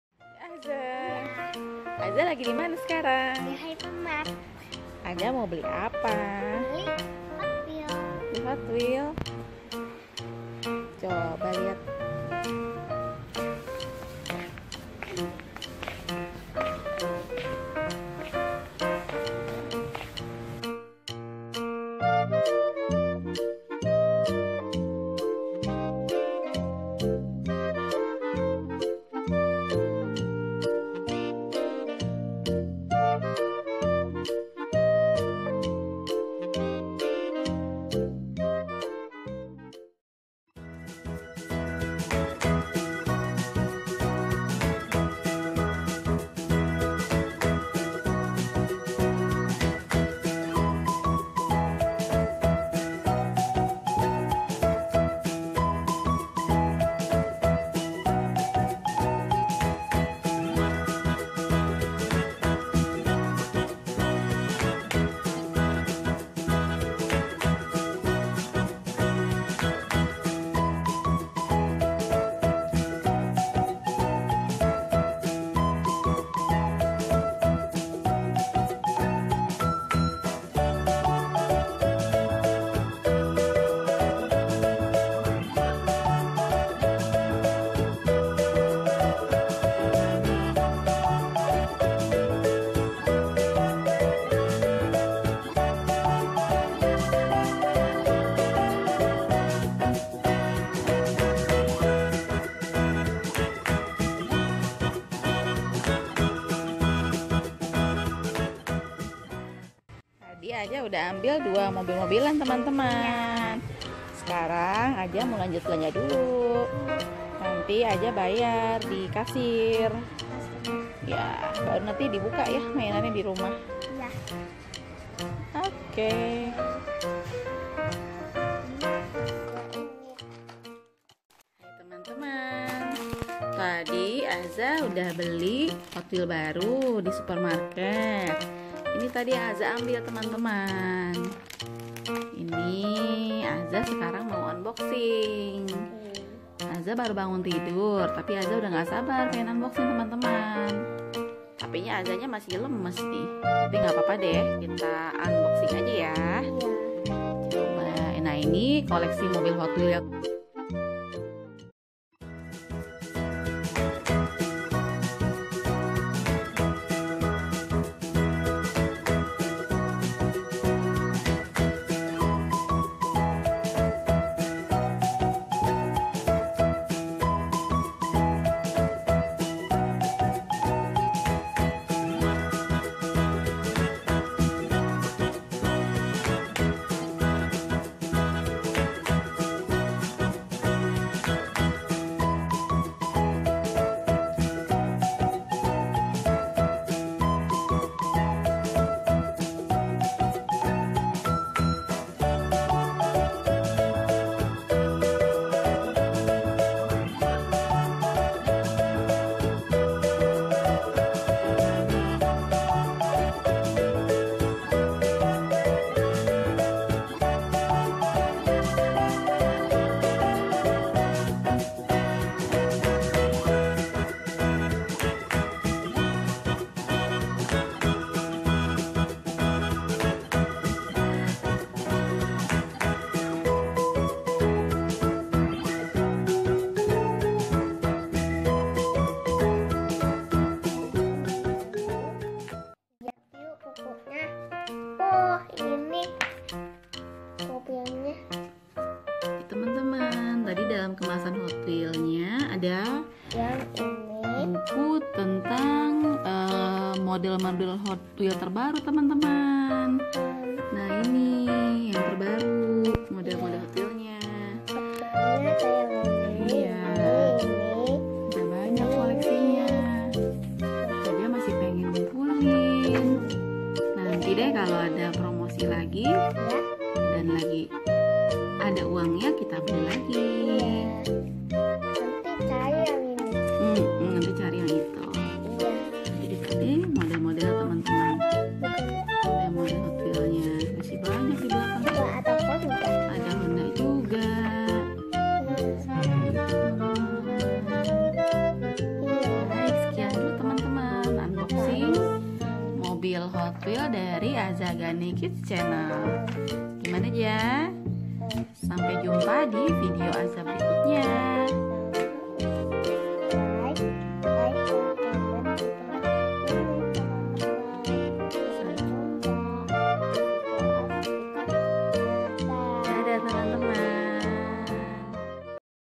Beli Mainan Mobil Hot Wheels dan Buka Mainan | Belanja Bulanan di Supermarket
Mau lihat seperti apa keseruannya? Yuk langsung aja kita lihat videonya teman-teman.